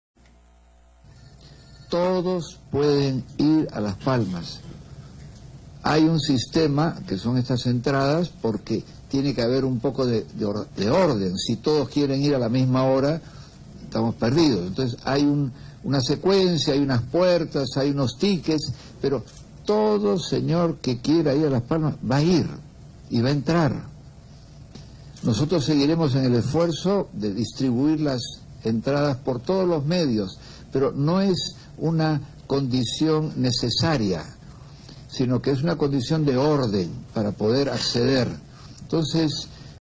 CIPRIANI: CUALQUIERA PUEDE IR A VER AL PAPA FRANCISCO
NO HABRA RESTRICCIONES SEGUN ESTE CARDENAL